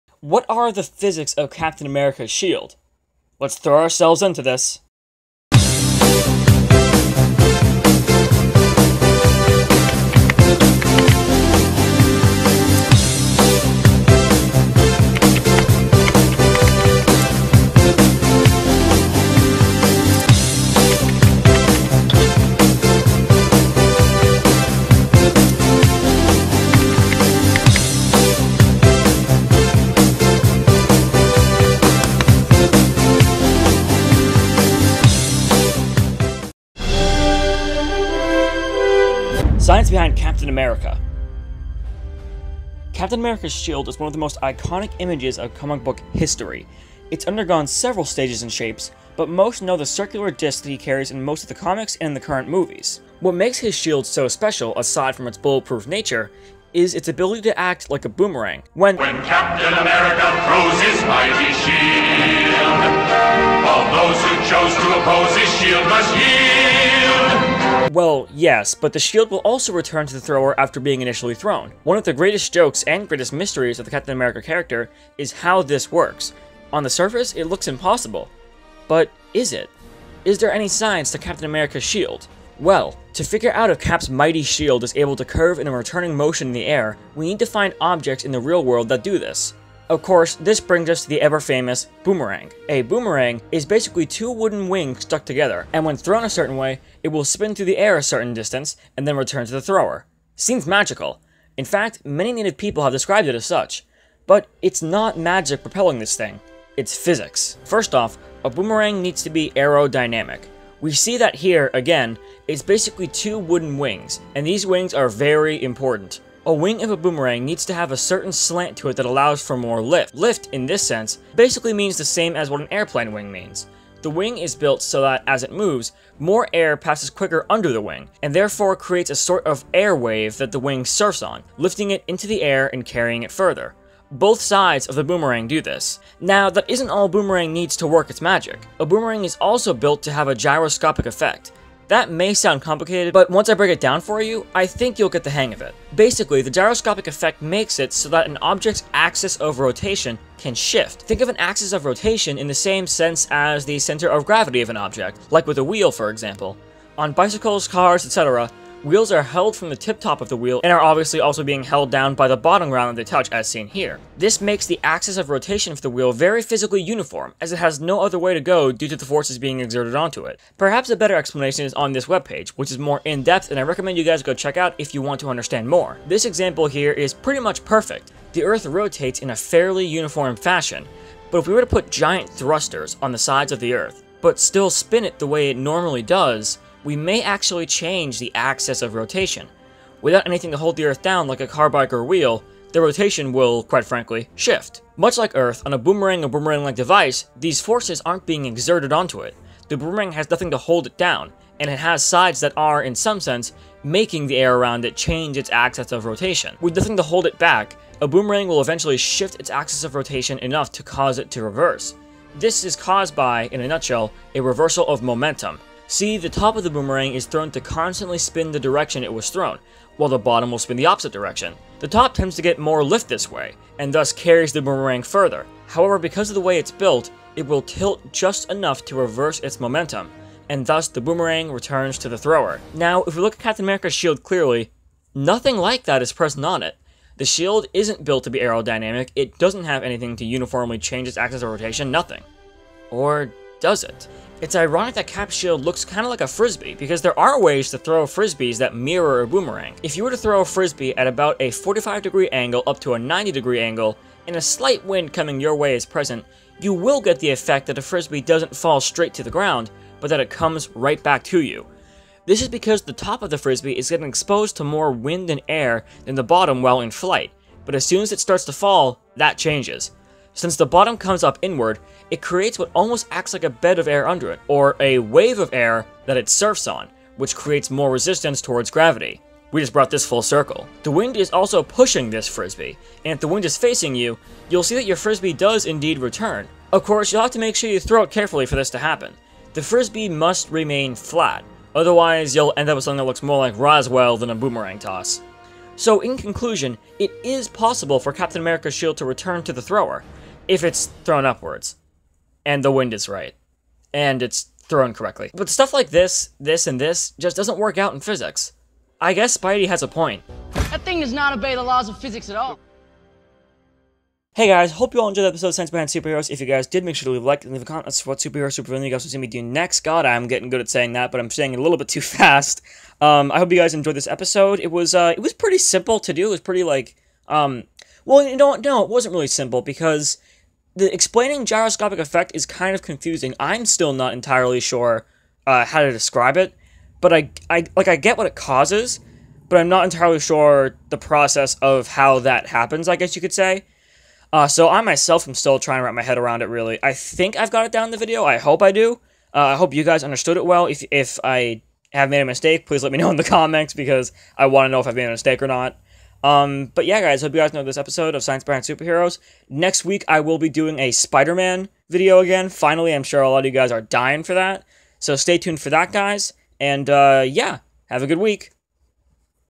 How Does CAPTAIN AMERICA'S SHIELD Work? - Science Behind Superheroes
How does Captain America's shield work? Let's throw ourselves into this!